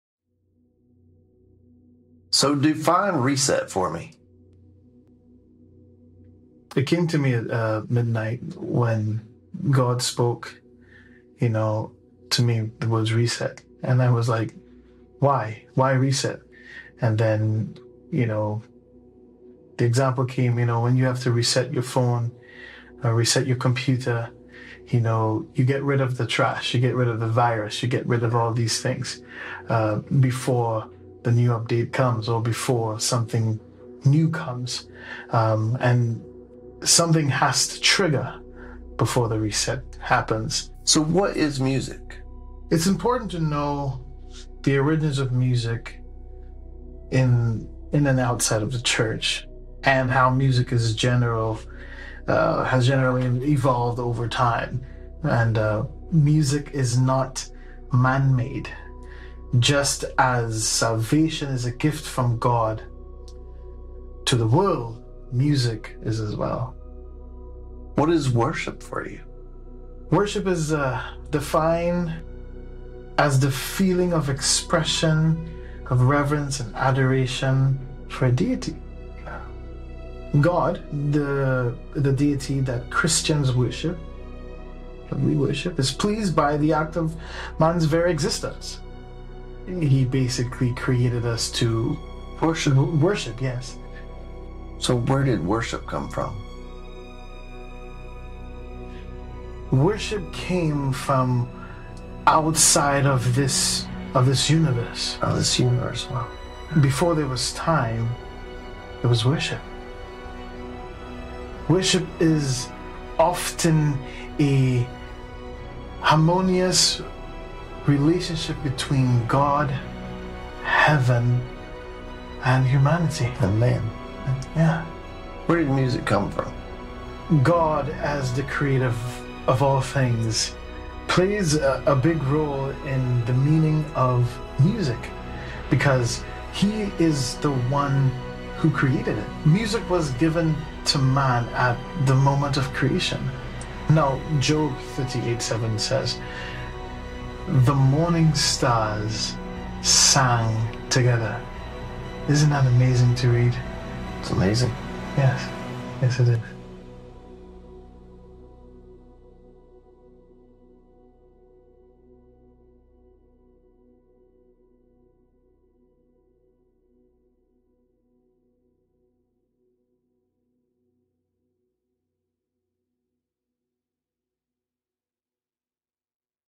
Where did music come from | Interview with Joshua Ali
Jevin Morris 🇺🇸 (from America) and I 🇹🇹 had a serious interview. These are the things that was discussed;
- Define RESET
- What is worship
- What is music
- Where did worship come from
- Where did music come from
Filmed by J Modeste